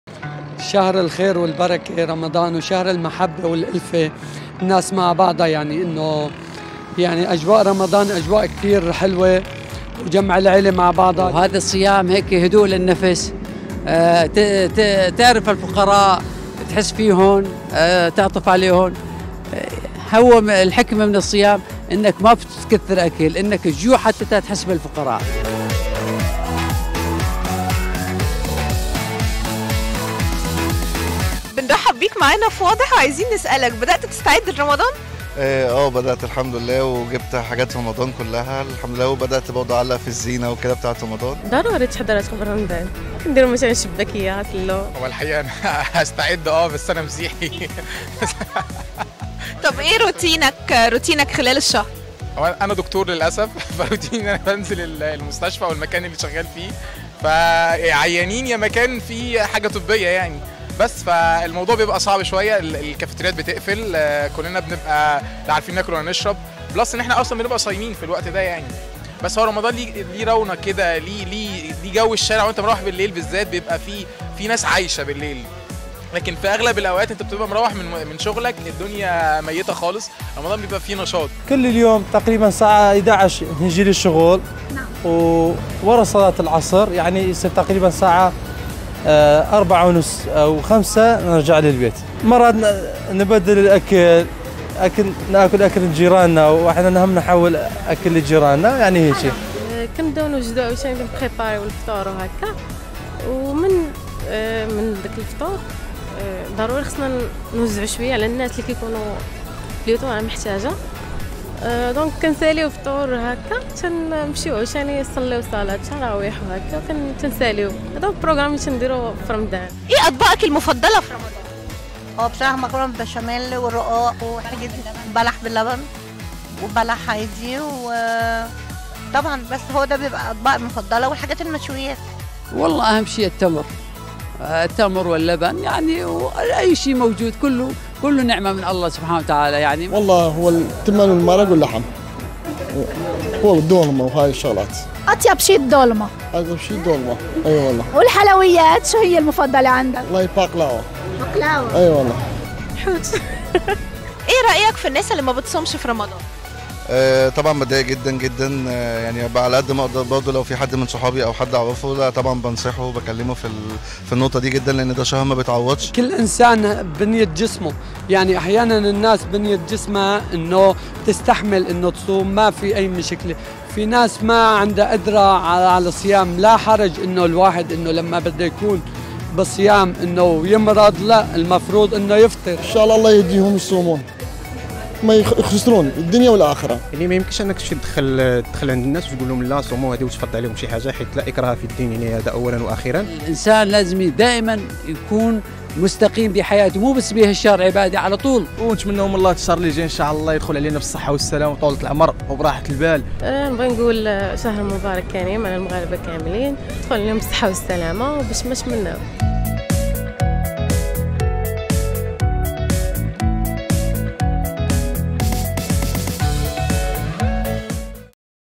Les préparatifs de Ramadan
Le Ramadan approche à grands pas! Avez-vous commencé les préparatifs ? Quel est votre plat préféré pendant ce mois ? Wada7 est sorti dans les rues du Maroc, l'Égypte, la Syrie et l'Irak pour discuter